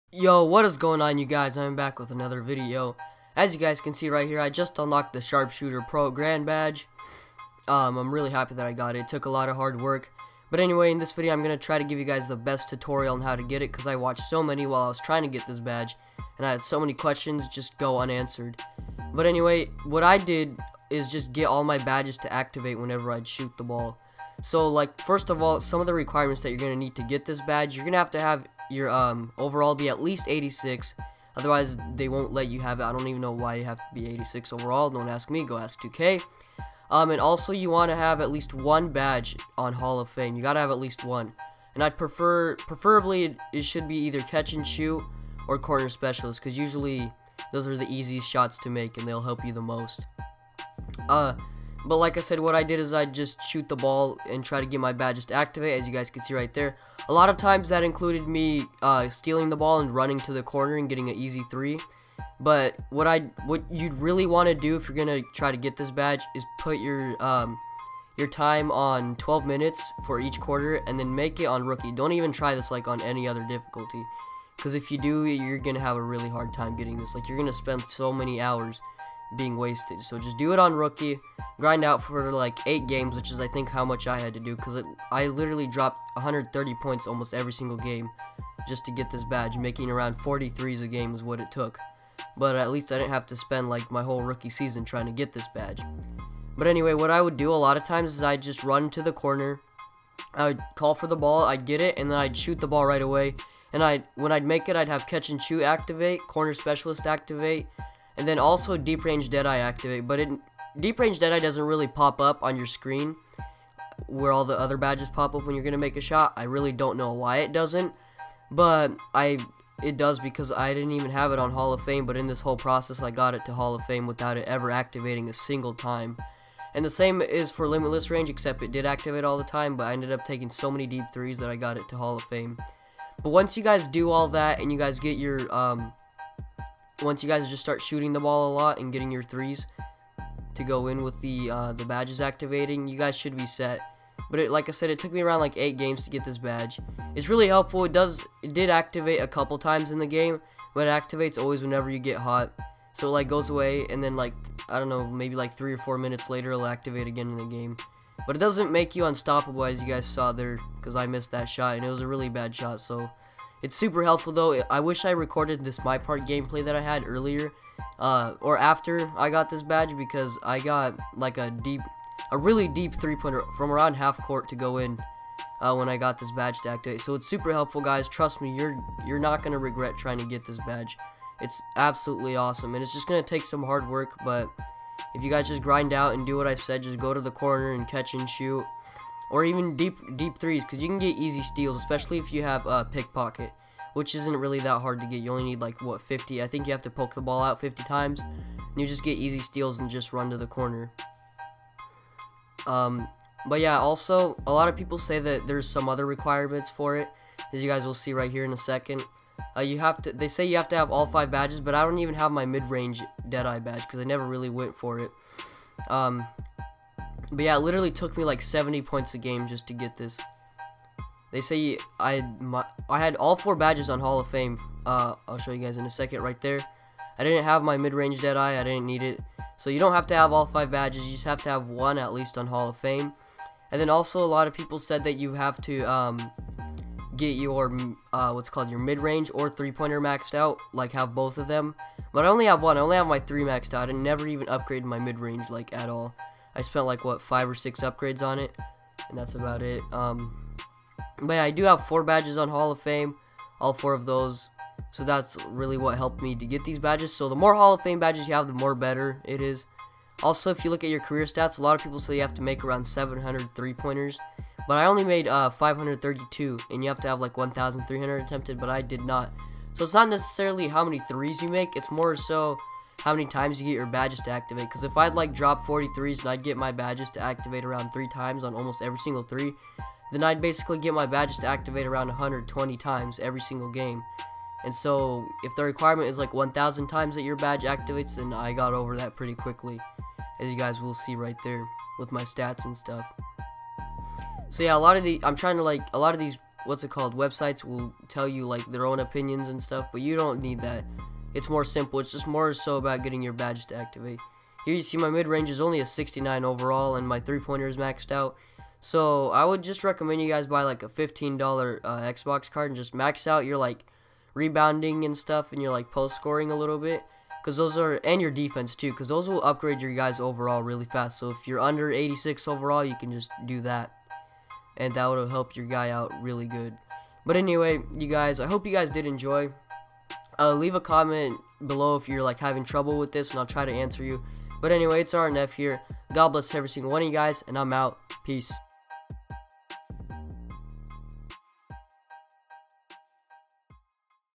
HOW TO GET THE SHARPSHOOTER GRAND BADGE! (BEST TUTORIAL)
This is how I got the Sharpshooter Pro Grand Badge. I think this is the best tutorial because I covered everything that I had trouble figuring out with when I was getting this badge.

God Bless all of you guys!